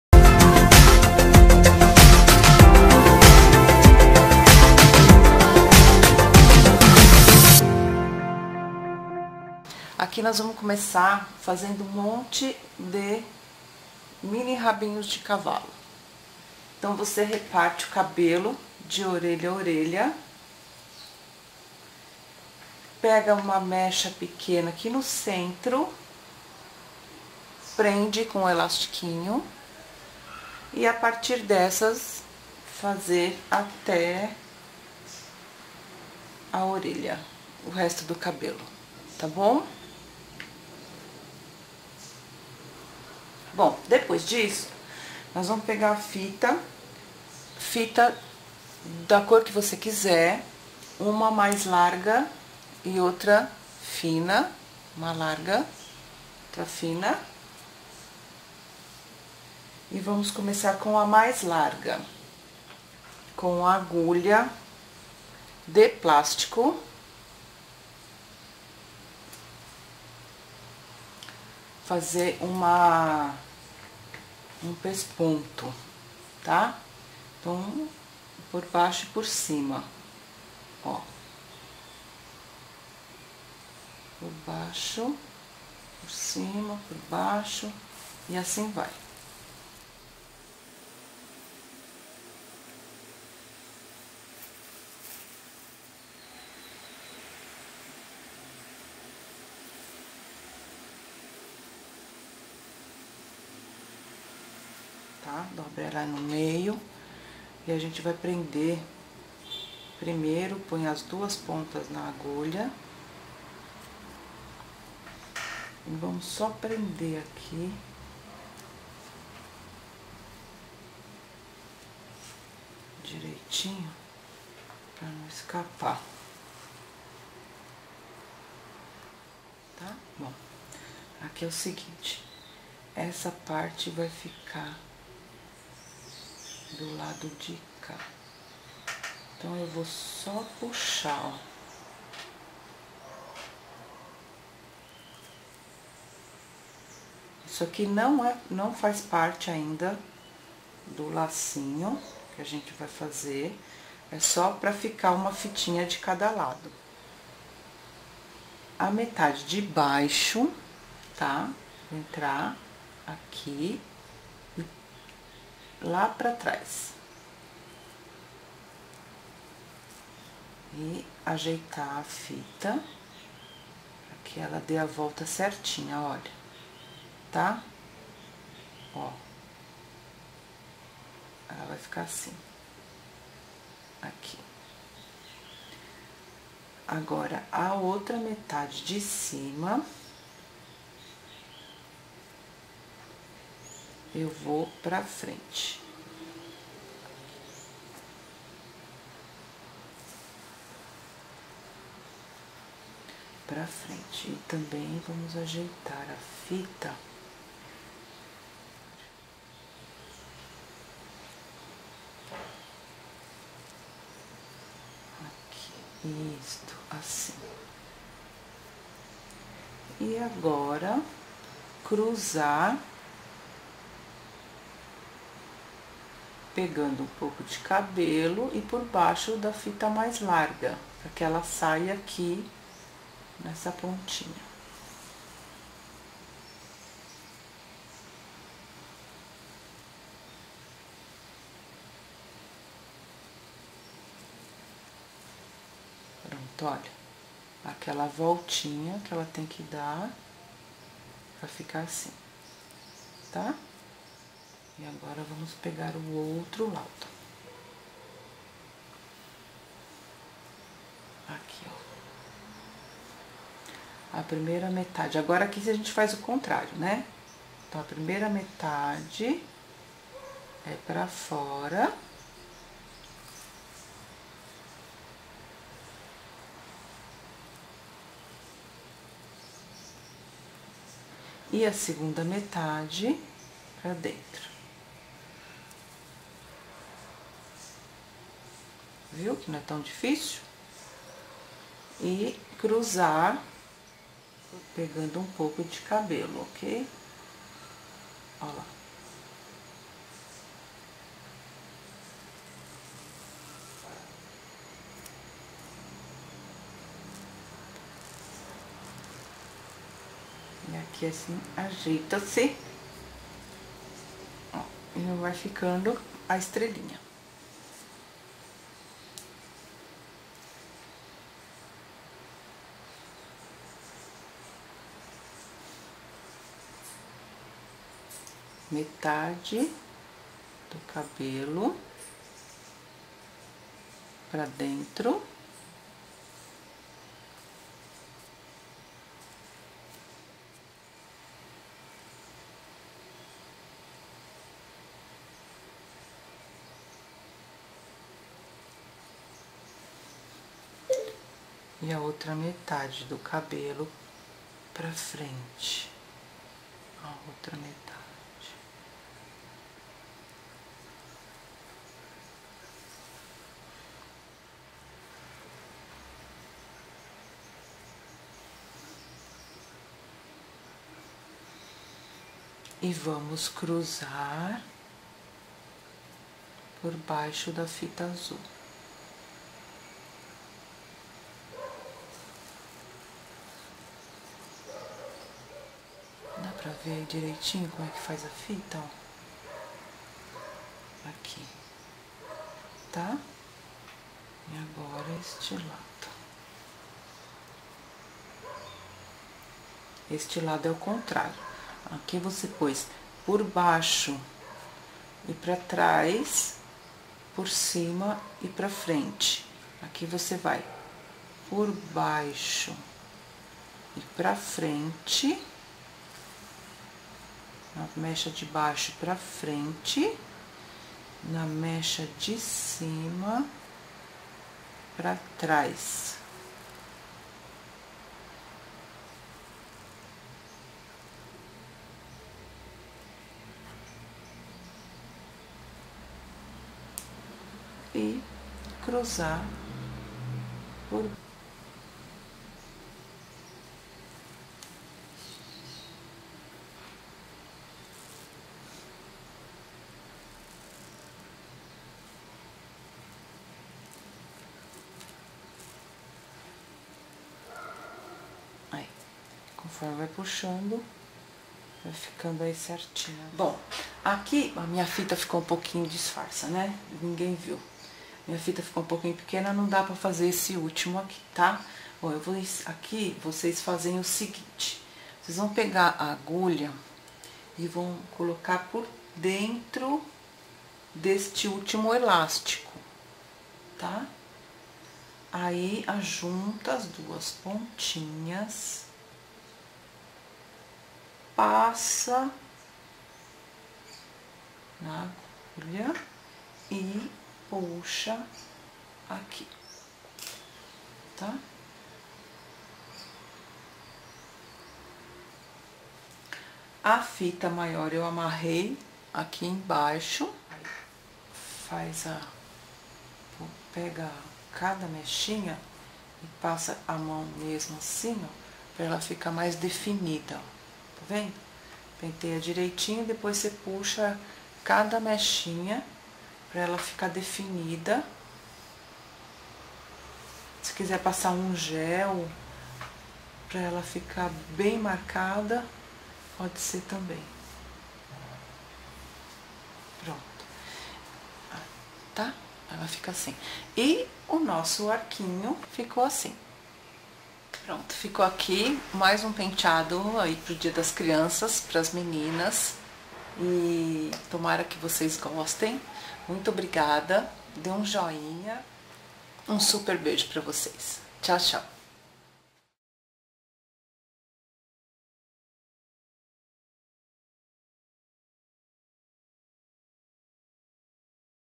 Trança para Criança, ribbon headband - Telma tranças
passo a passo de trança para criança com fitas, uma linda tiara de elásticos e fitas coloridas.
paso a paso trenza con cinta para niñas.
how to make a ribbon headband for girls.

google+: goo.gl/S6JNPP

telmatranças @telmatrancasoficial @elianaoficial @galisteuoficial جمال